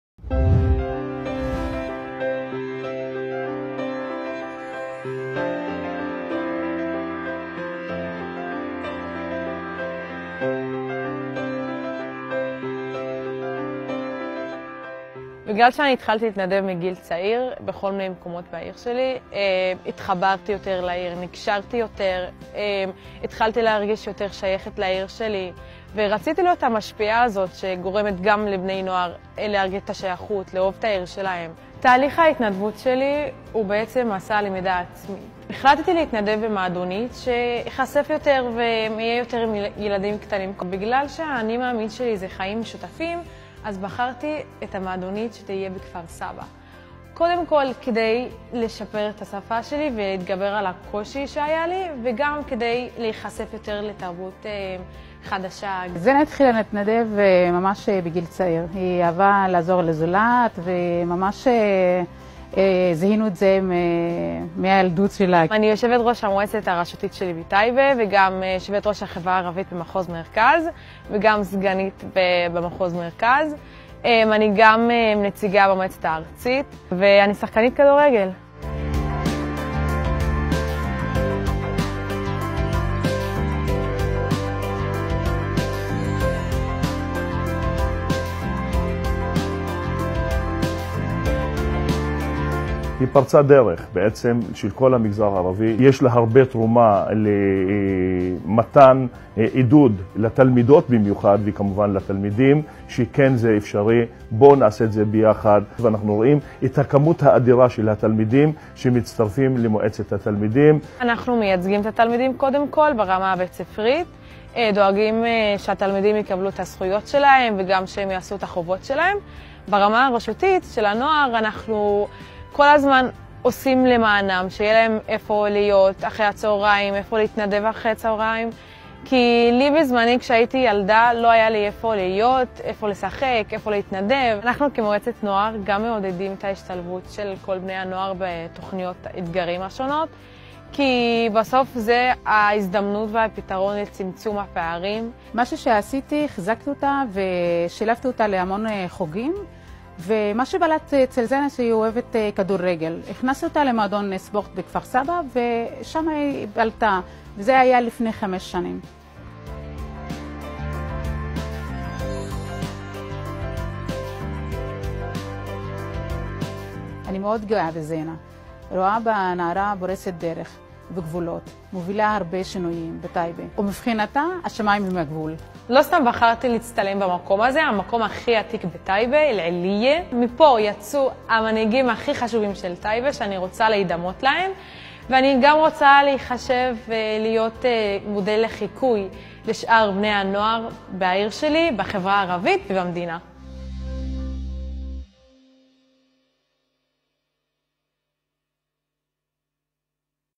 تعرفوا على الطالبة زينة حاج يحيى من الطيبة الحائزة على جائزة التربية
تعرفوا على الطالبة زينة حاج يحيى من مدرسة عتيد الطّيبة الحاصلة على الجائزة الخاصّة من وزير التربية على تطوّعها من أجل المجتمع .
جائزة وزير التّربية للشّبّيبة المتفوّقة على اسم المرحوم العاد ريبن